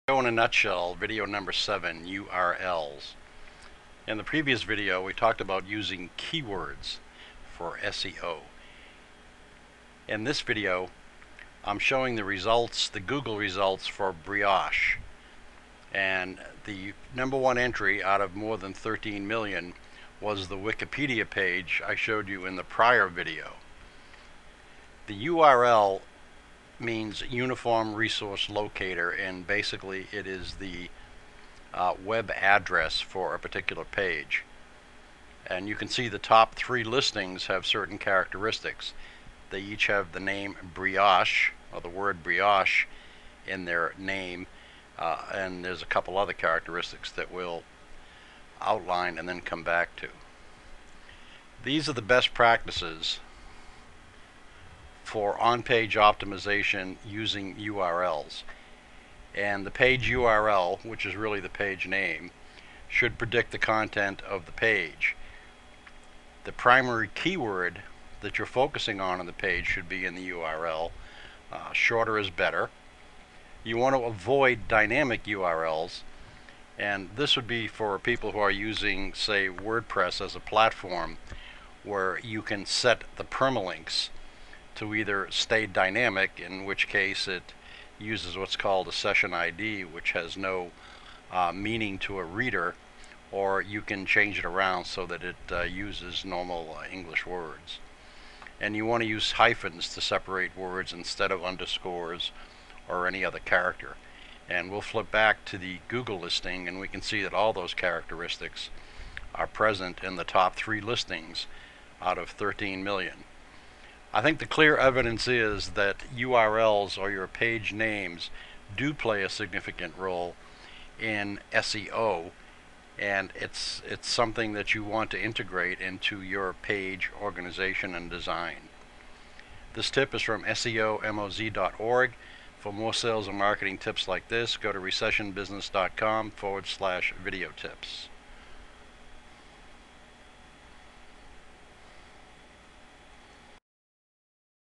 SEO in a Nutshell #7
This video series explains what SEO (search engine optimization) is and why it is important.